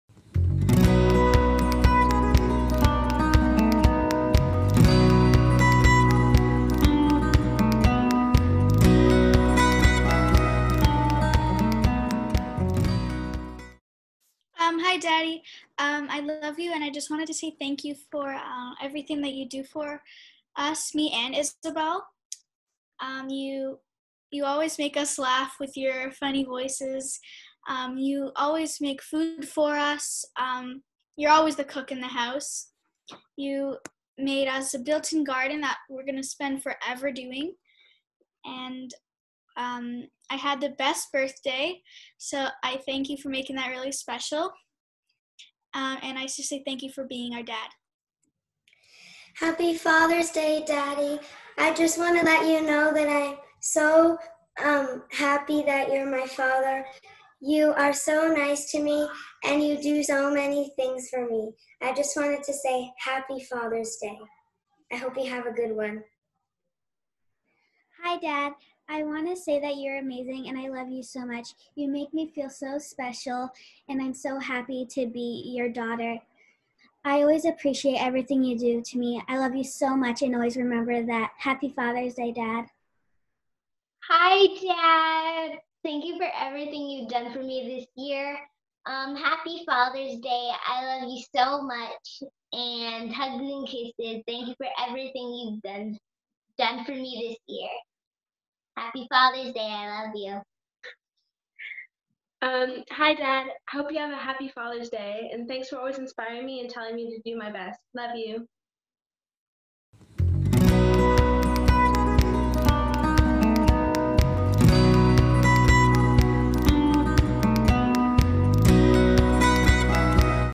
Happy Fathers Day - Marquee's Vocal Class
Some of Marquee's Group Vocal class students wanted to say Happy Fathers Day!